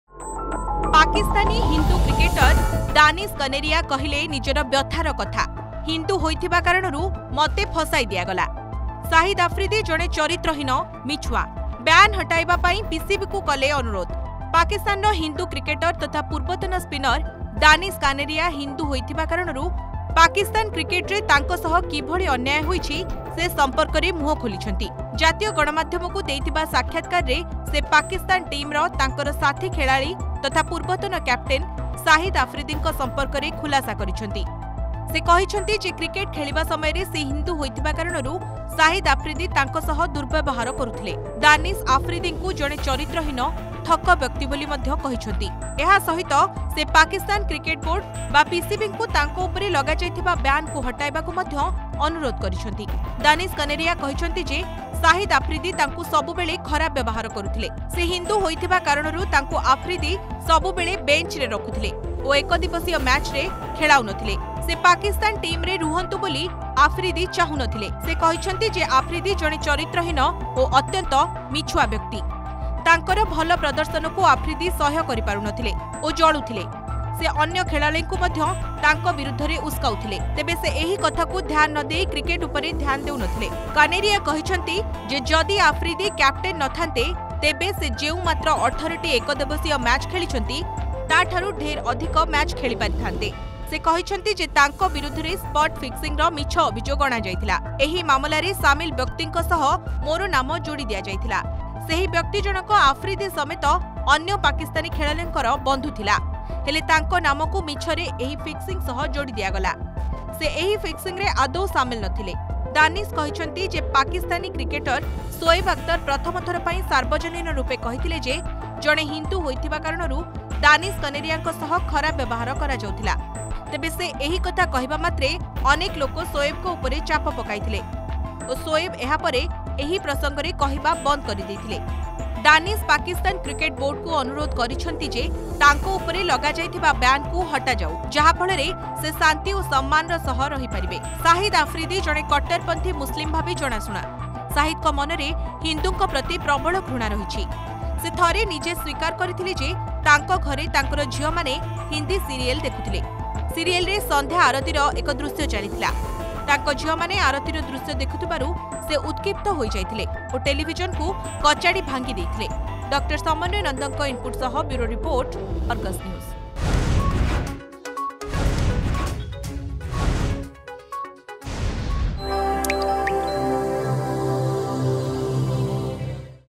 ପାକିସ୍ତାନୀ ହିନ୍ଦୁ କ୍ରିକେଟର ଦାନିଶ କନେରିଆ କହିଲେ ନିଜର ବ୍ୟଥା କଥା
ପାକିସ୍ତାନର ହିନ୍ଦୁ କ୍ରିକେଟର ତଥା ପୂର୍ବତନ ସ୍ପିନର ଦାନିଶ କାନେରିଆ ହିନ୍ଦୁ ହୋଇଥିବା କାରଣରୁ ପାକିସ୍ତାନ କ୍ରିକେଟରେ ତାଙ୍କ ସହ କିଭଳି ଅନ୍ୟାୟ ହୋଇଛି ସେ ସମ୍ପର୍କରେ ମୁହଁ ଖୋଲିଛନ୍ତି